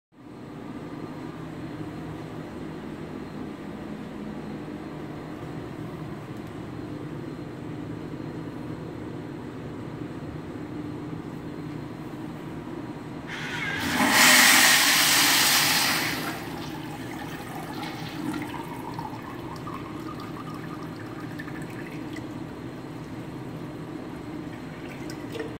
254: Kohler Darfield
This urinal was located in a corner of a decently dark locker room(sorry for the fuzziness).I think my new phone handled the darkness pretty well. It looks much brighter in the video than it actually was.

 I recorded it after school so nobody would walk in and ask what I was doing lol! This is definitely my favorite modern washout urinal of the non internal trap variety.